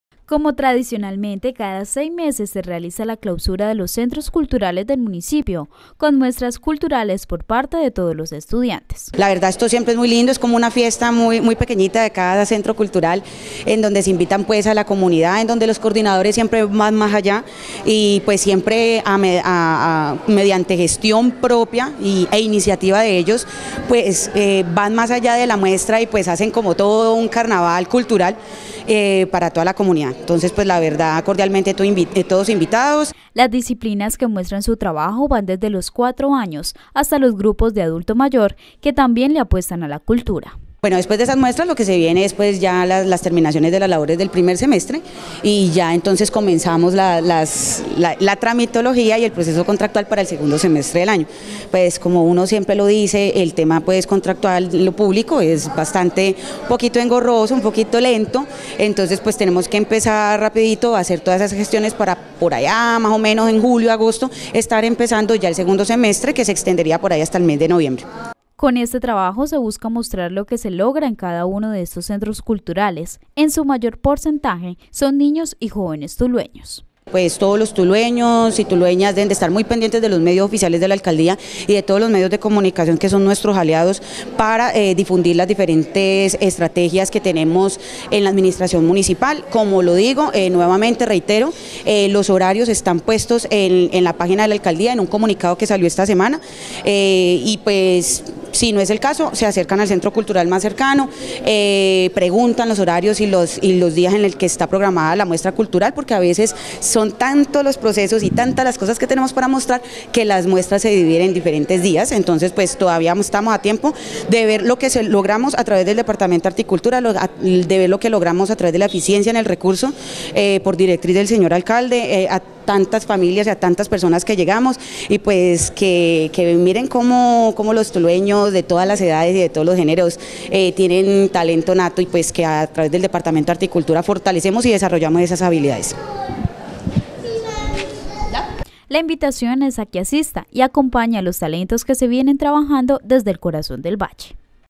CNC Noticias Tuluá quiere felicitar a los Abogados
CNC Noticias Tuluá quiere felicitar a los Abogados del Municipio, que gracias a esa importante labor